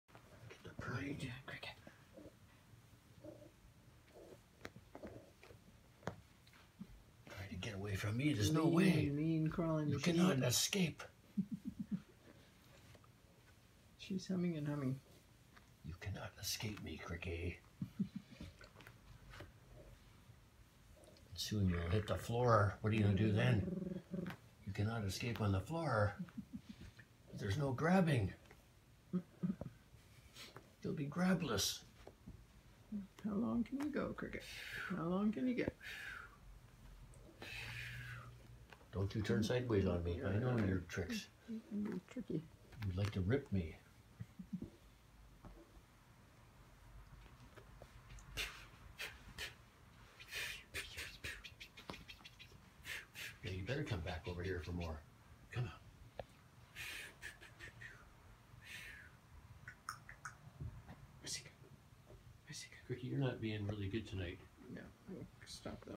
Crickie gets thrashed IMG 1135
Her nightly game on our living room floor...She comes in and flops in front of me and i get down on the floor give her very vigorous back rubs as she pulls her way across the rug. When she gets to the hardwood i go back to my chair and she comes back for more. purring very loudly the whole